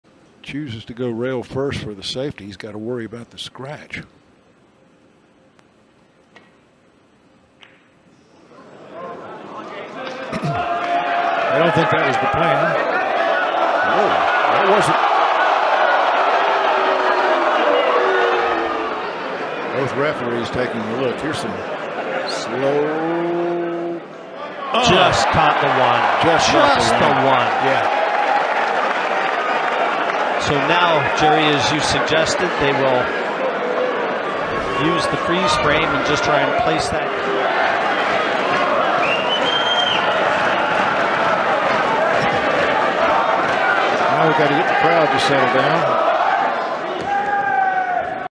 Mosconi Cup Drama Referee Mistake | Mosconi Cup 2019
Mosconi Cup Drama Referee Mistake | Mosconi Cup 2019 Jayson Shaw Vs Billy Thorpe